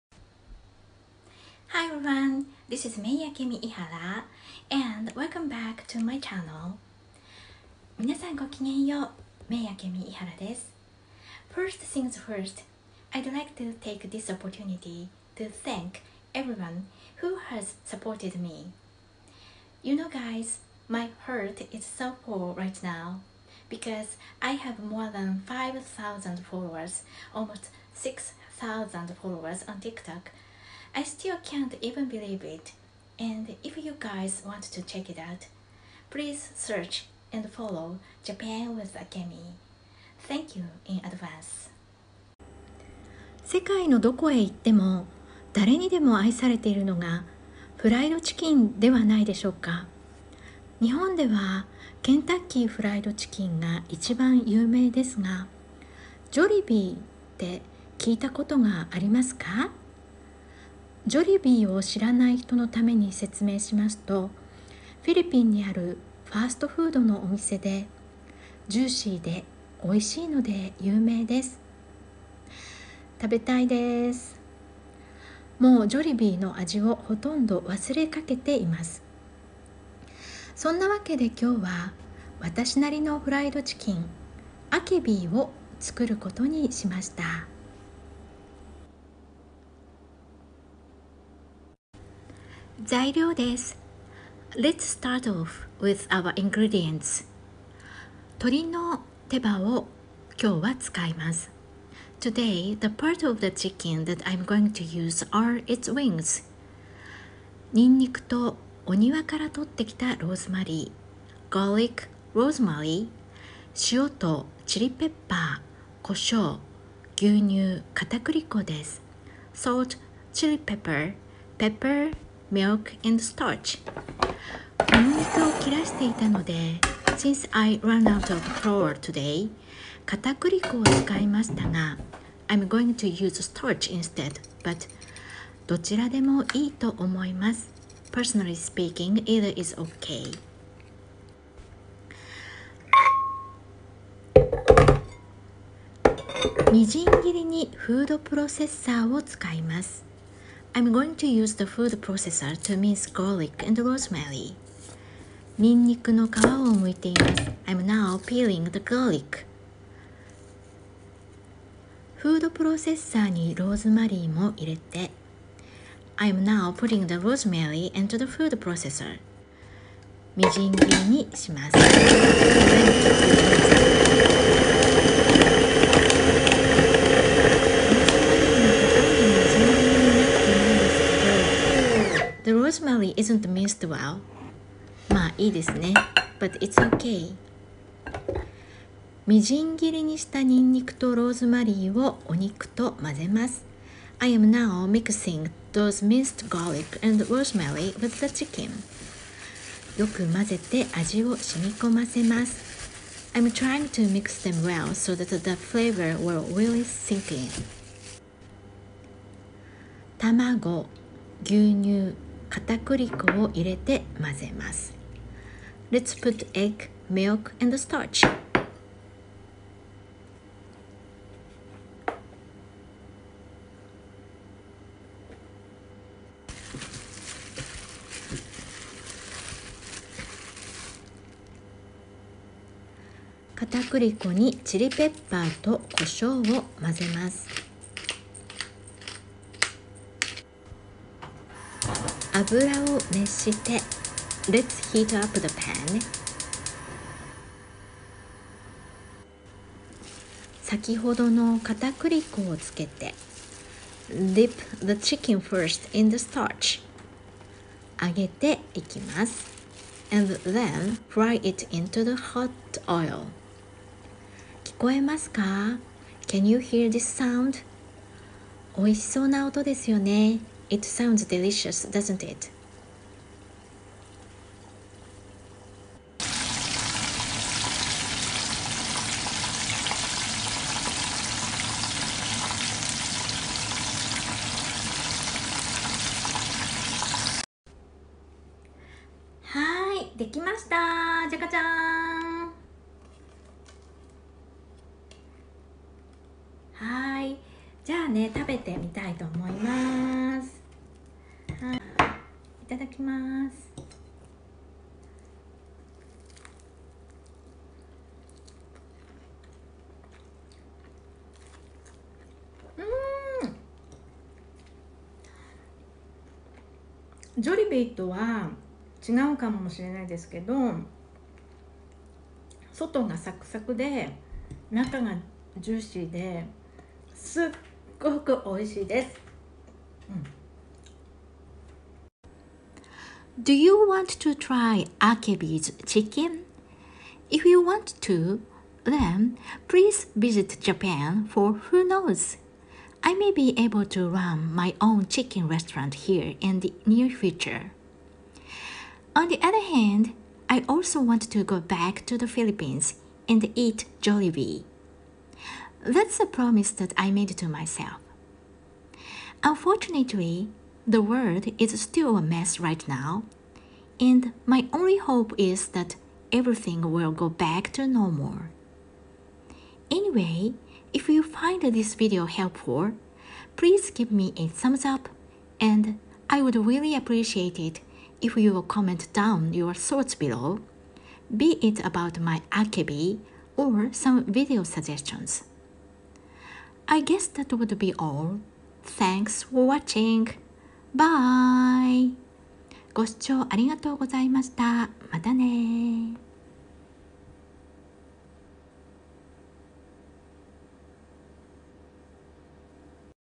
Jolibee? KFC? No, Akebee Fried chicken!
Darenidemo aisareteirunoga
だれにでも愛されているのが
there's one dish that is loved by everyone and

Fired chicken dewa naidesyouka
フライドチキンではないでしょうか
 that is no other than, fried chicken.

Nihon dewa日本ではIn behalf of Japanese people,

KFCga ichiban yuumei desuga
ケンタッキー・フライド・チキンがいちばん有名ですが
I would say KFC is the most famous fast food chicken chain here but

Jolibee tte kiitakotga arimasuka
ジョリビーって聞いたことがありますか
Have you guys heard about Jollibee?

Jolibee o shiranai hito no tameni setsumei shimasuto
ジョリビーを知らない人のために説明しますと
For those who don't know,

Philippines ni aru fast food no omise de
フィリピンにあるファーストフードのお店で
Jollibee is a multi-national chain of fast food restaurants in the Philippines and

Juicy de oishii node yuumei desu
ジューシーで美味しいので有名です
they're famous for their ever juicy Chickenjoy.

,tabetai desu
食べたいです
I badly miss it and

Mou Jolibee no aji o hotondo wasure kakete imasu
もうジョリビーの味をほとんど忘れかけています
it seems like I already forgot its taste.

Sonna wakede kyouwa
そんなわけで今日は
That's why for today's video,

Watashi narino fried chicken,Akebee o tsukuru kotoni shimashita
私なりのフライドチキン、アケビーを作ることにしました.
I'd like to make my own version of it and it's called Aka-bee (laugh).　４０